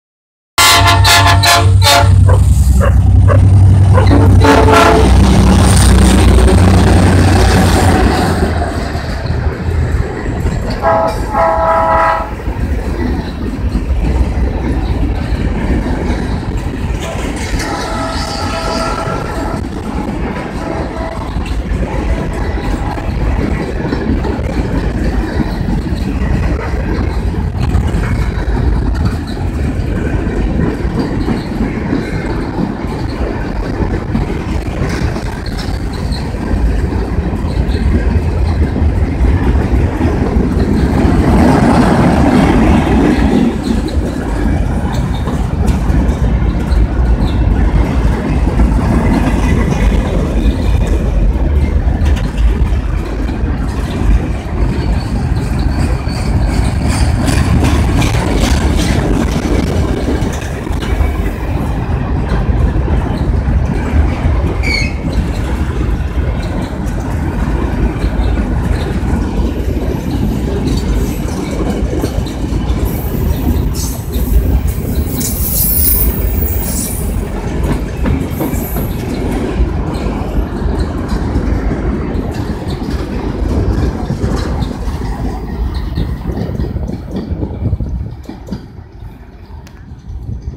Tren vacio de NCA rumbo a Dalmacio Velez Sarsfield
En la mañana de hoy y con el Sol brillando a pleno en el cielo azul de tan hermoso día, las EMD GT-22CW 9097 y GR-12W 6614 con un tren vacio compuesto por 18 portacontenedores y 30 tolvas graneras hacían su paso por aquí por la Ciudad de Marcos Juárez rumbo a Dalmacio Velez Sarsfield. Marcos Juárez. Córdoba. Argentina. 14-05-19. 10:00 a.m.